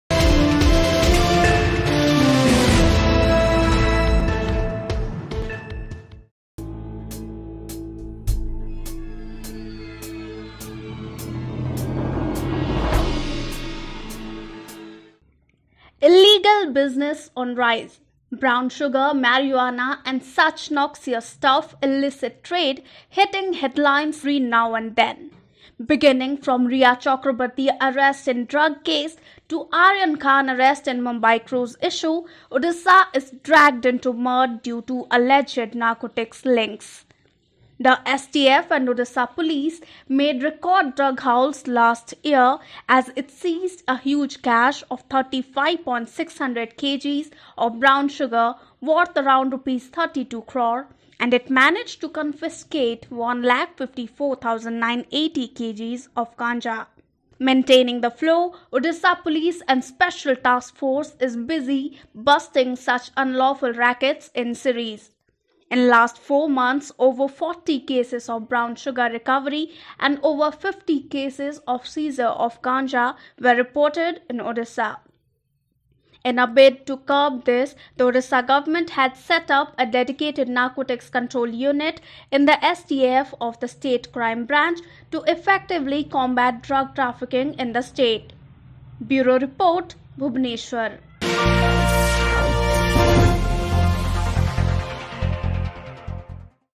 Drug dragging Odisha I Brown sugar, Marijuana and such noxious stuff illicit tradehitting headlines
Illegal business on rise
Brown sugar, Marijuana and such noxious stuff illicit trade
hitting headlines every now n then.
Beginning from Rhea Chakraborty arrest in drug case to Aryan
Khan arrest in Mumbai cruise issue, Odisha is dragged into mud
due to alleged narcotics links.
The STF and Odisha Police made record drug hauls last year as
it seized a huge cache of 35.600 kgs of brown sugar worth
around Rs 32 crore and it managed to confiscate 1,54,980 kg of
ganja.
Maintaining the Flow, Odisha Police and Special Task Force is
busy busting such unlawful rackets in series.
In last 4 months over 40 cases of Brown sugar recovery and over
50 cases of seizure of Ganja were reported in Odisha